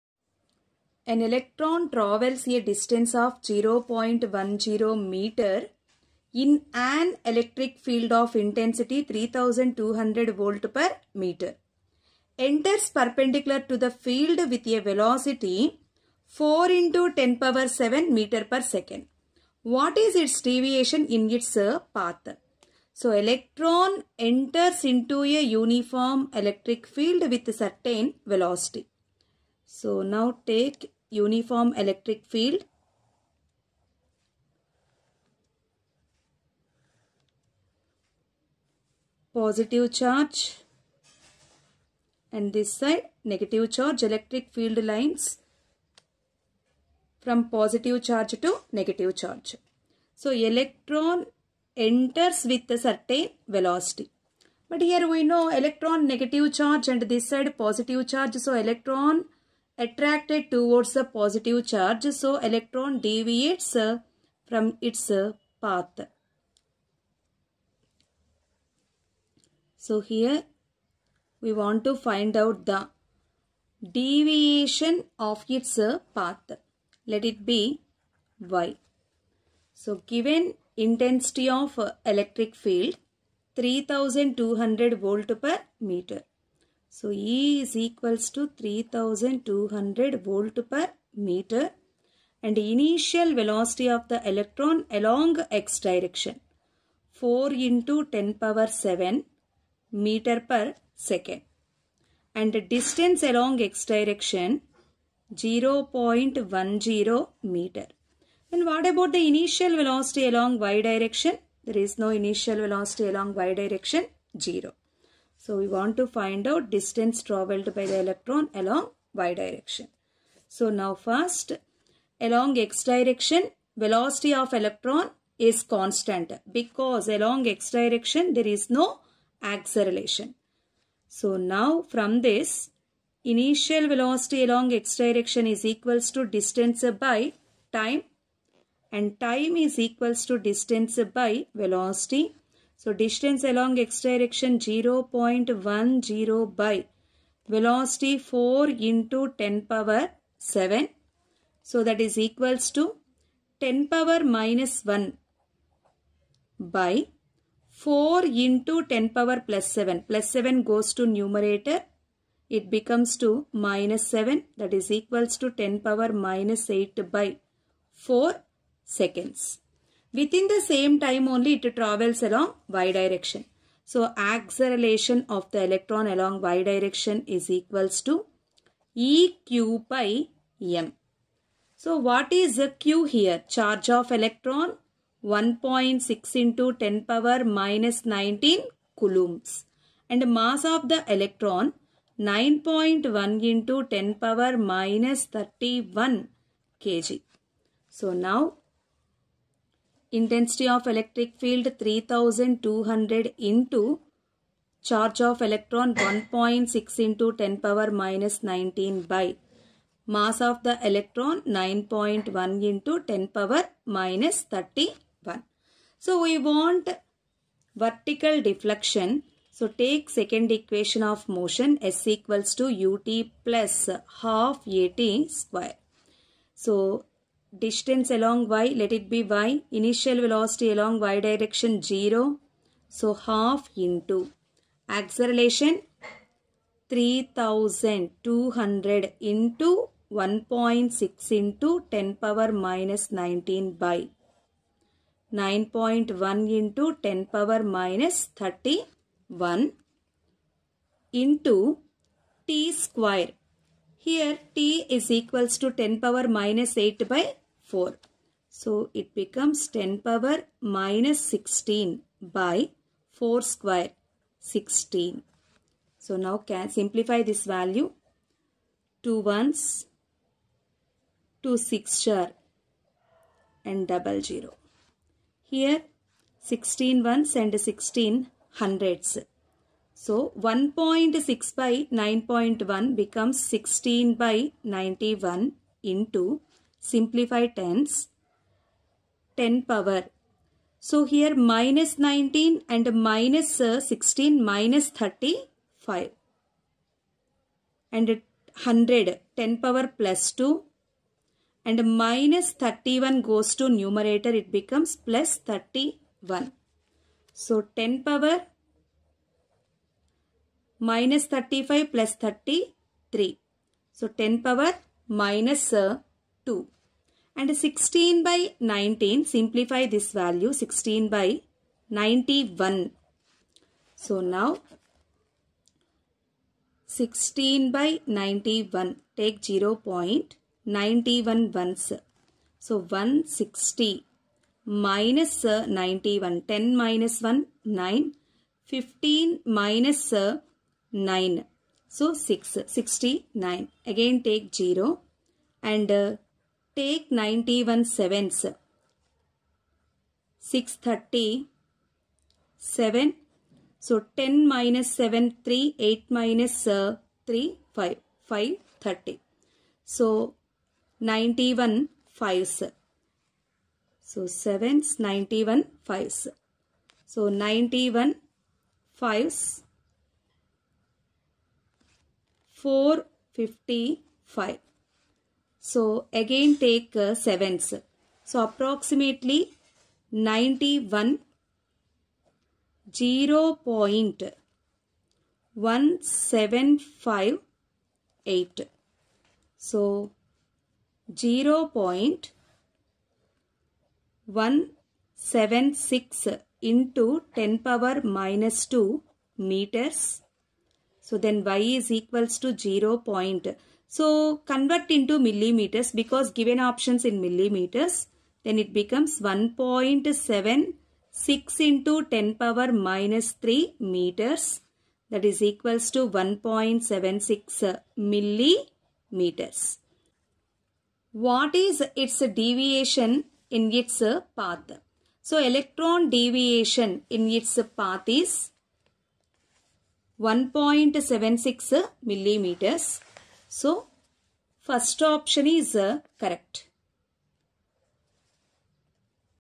an electron travels a distance of 0.10m in an electric field of intensity 3200v/m enters perpendicul
an electron travels a distance of 0.10m in an electric field of intensity 3200v/m,enters perpendicular to the field with a velocity 4×10^7m/s,what is it's deviation units path
electrostatics class 12 physics
numericals on electrostatics